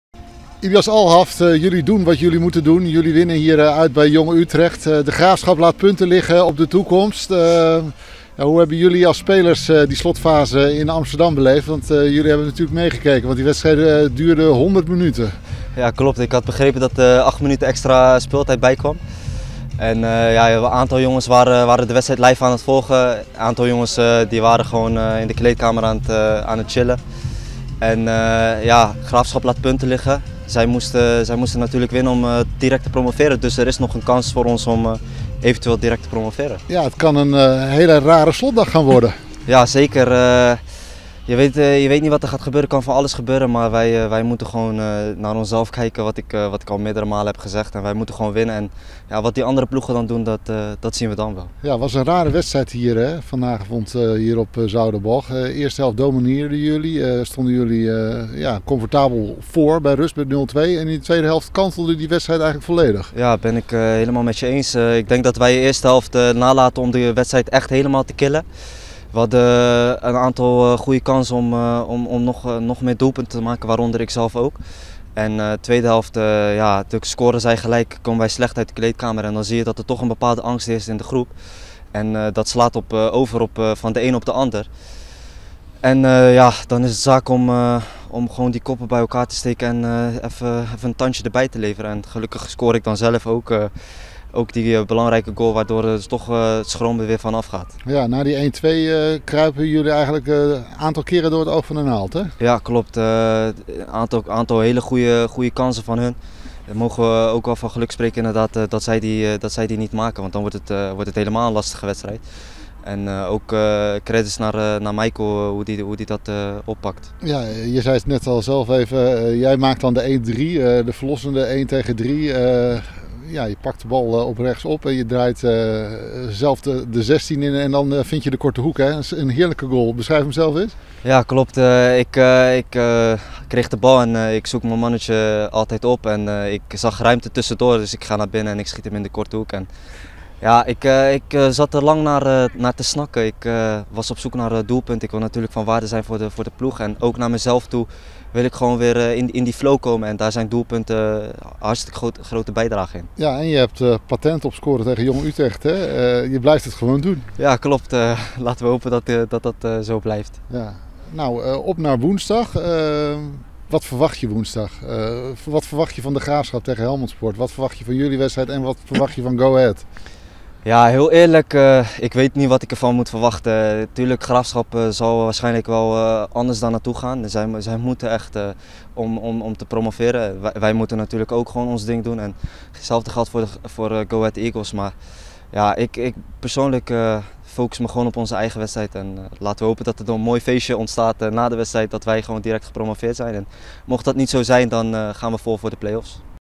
Ilias Alhaft van Almere City FC.
Ilias Alhaft blikt terug op de wedsrtijd Jong Utrecht - Almere City FC. De Almeerders wonnen met 1-3 en maken nog kans op rechtstreekse promotie.